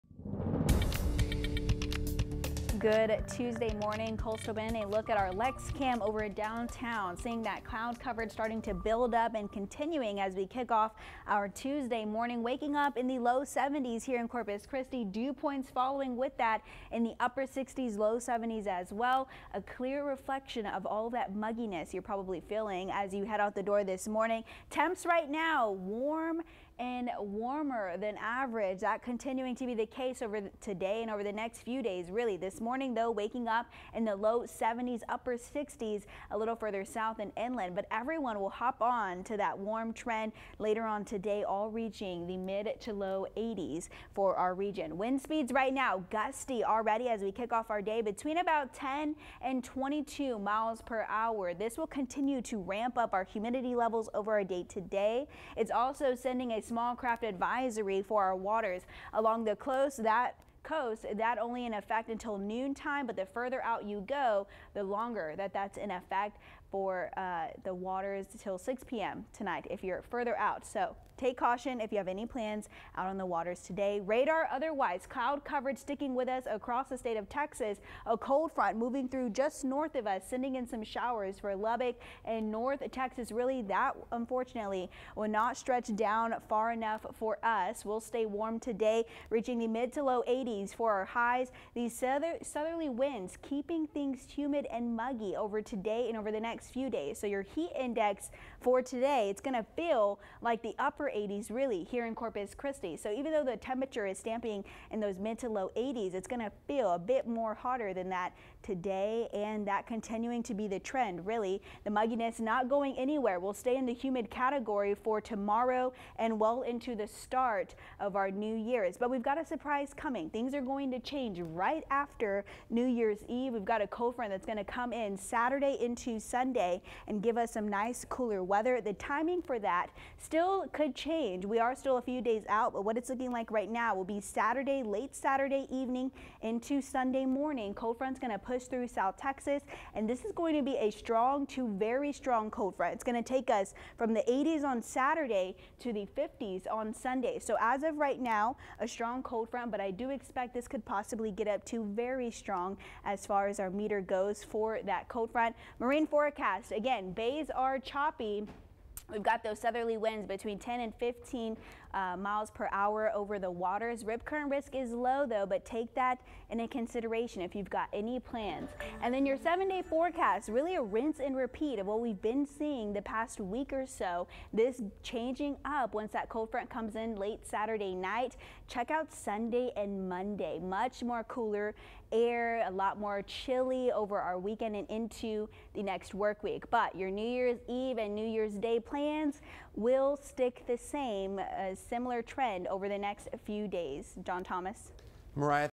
Tuesday forecast: Warm, humid and breezy in Corpus Christi
High temperatures stay in the middle-80s through the rest of the week. This ahead of a weekend cold front.